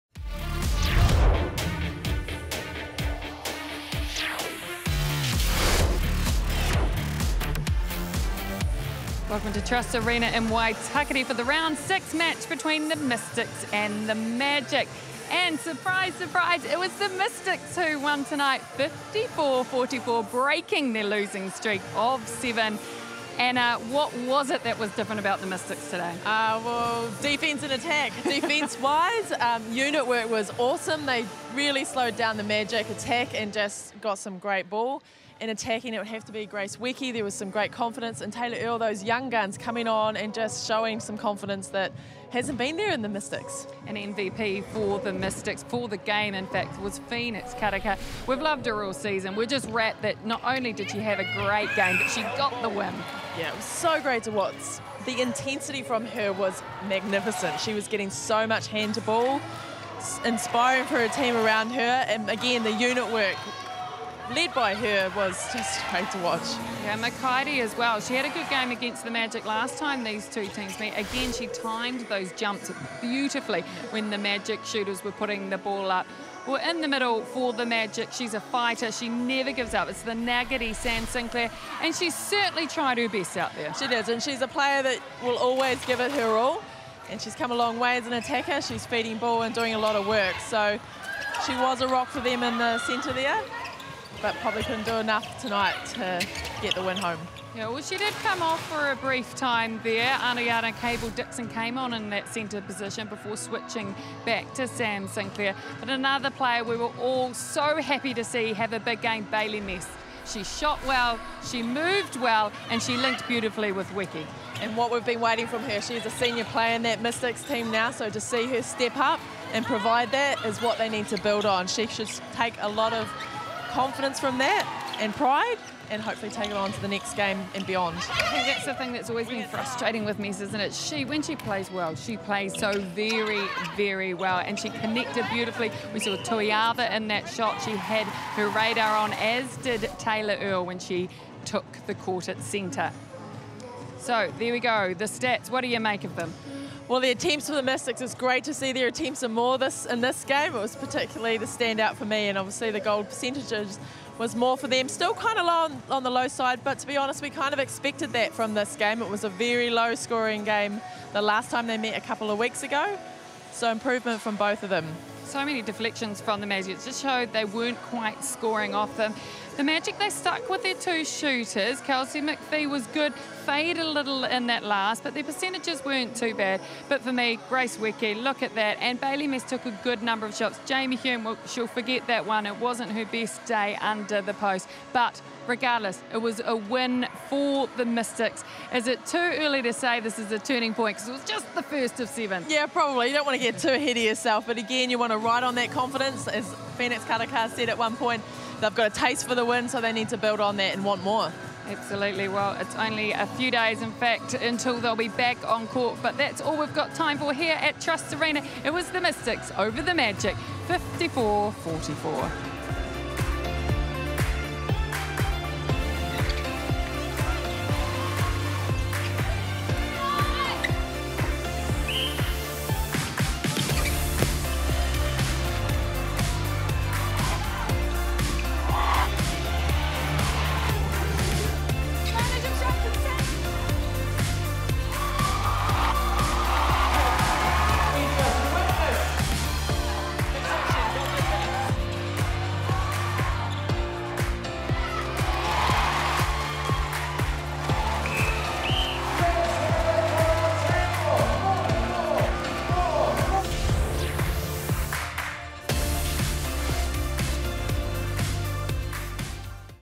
2019 ANZ Premiership | Round 6 - Mystics v Magic
Watch SKY Sport's wrap of the Round 6 clash between the Mystics and Magic from Auckland.